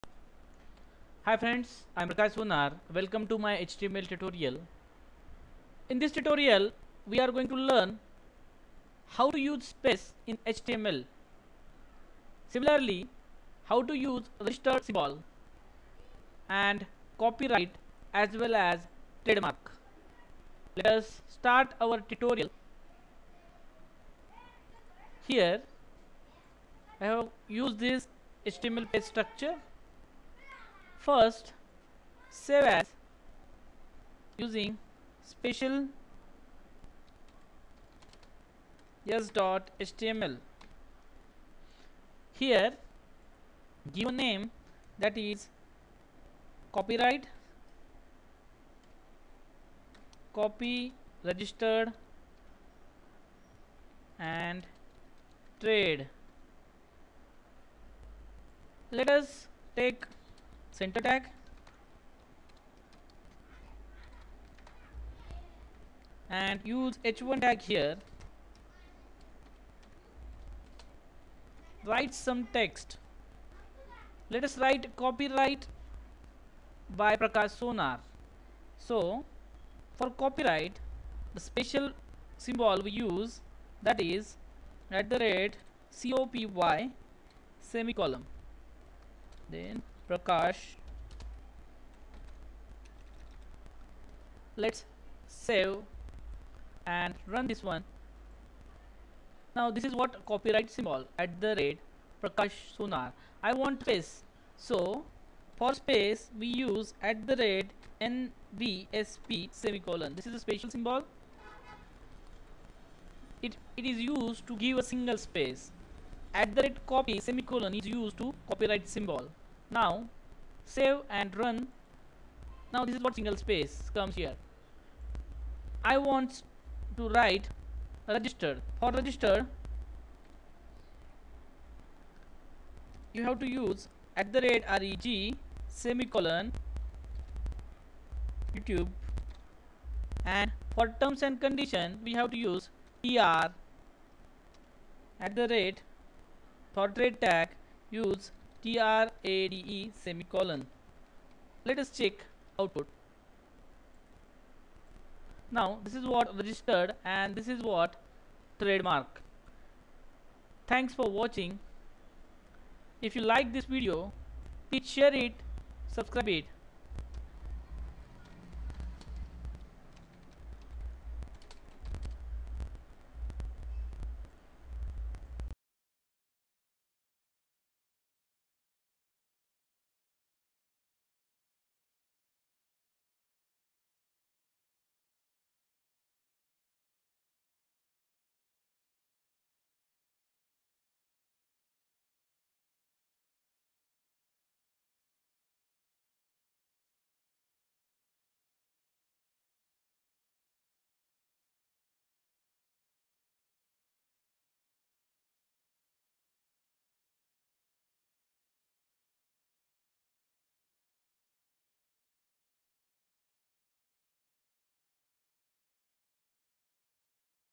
html copyright | html trademark | html registered | html space | special tag html
in this video tutorial you will learn how to use html spacial tag like html SPACE, html REGISTERED , html COPYRIGHT , html TRADEMARK tag